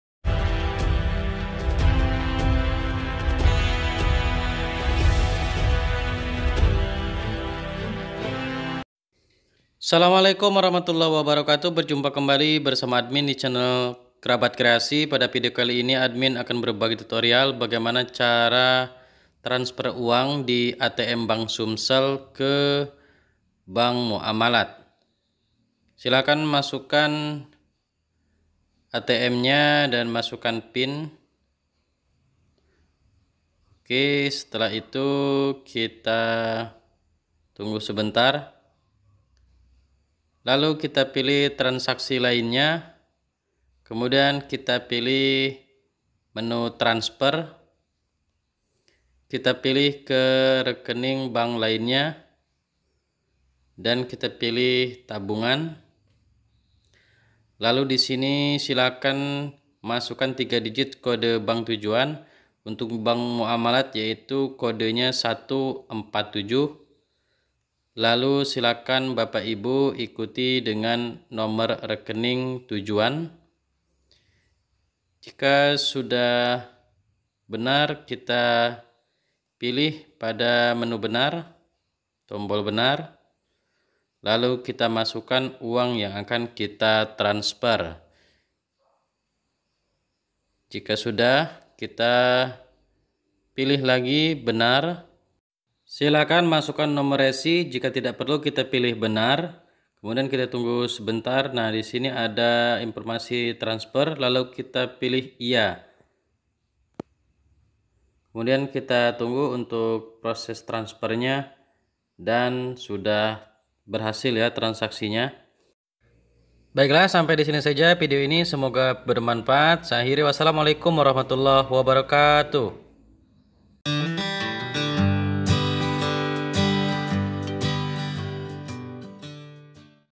CARA TRANSFER UANG DARI ATM BANK SUMSEL - BABEL KE BANK MUAMALAT
Cara kirim uang dari atm bank sumsel babel ke bank muamalat
Cara transfer uang dari atm bank sumsel babel ke muamalat